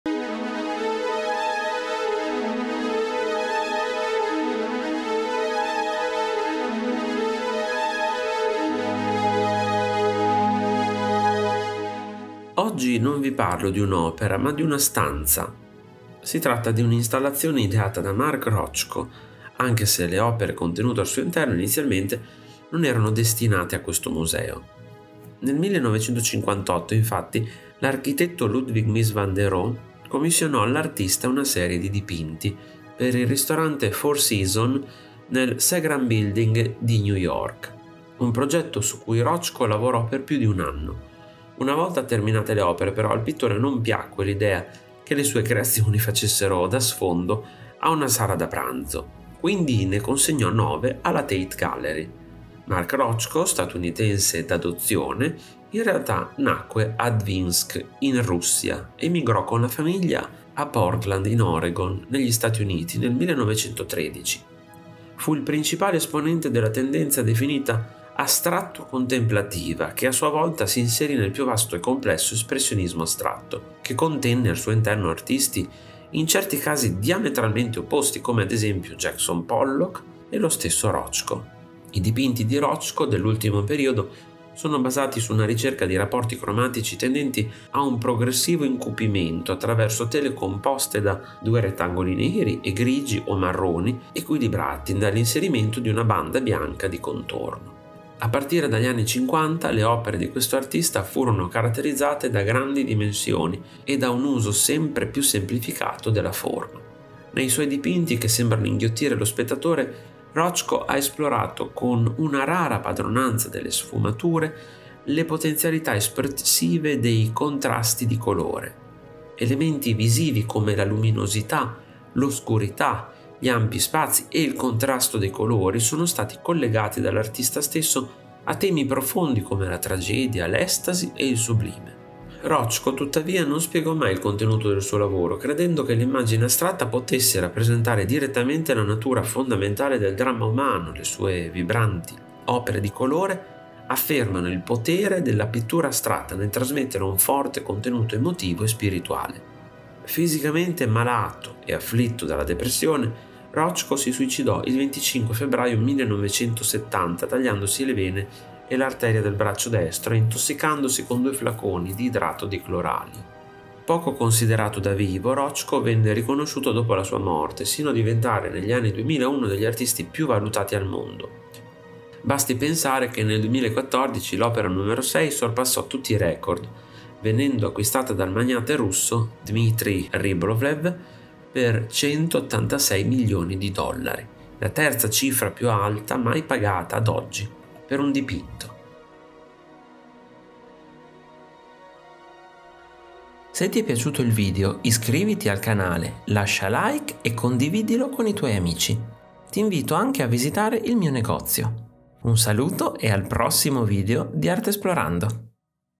Mark Rothko e la sala alla Tate Modern
L'audioquadro è un modo per conoscere i più grandi capolavori della storia dell'arte. In maniera semplice e in pochi minuti. Qui vi parlerò della sala alla Tate Modern di Mark Rothko▼▼CONTINUA SOTTO▼▼

Oggi non vi parlo di un’opera, ma di una stanza! Si tratta di un’istallazione ideata da Mark Rothko anche se le opere contenute al suo interno inizialmente non erano destinate a questo museo. Nel 1958 infatti l’architetto Ludwig Mies van der Rohe commissionò all’artista una serie di dipinti per il ristorante Four Seasons nel Seagram Building di New York. Un progetto su cui Rothko lavorò per più di un anno. Una volta terminate le opere però al pittore non piacque l’idea che le sue creazioni facessero da sfondo a una sala da pranzo. Quindi ne consegnò nove alla Tate Gallery.

LETTURE CONSIGLIATE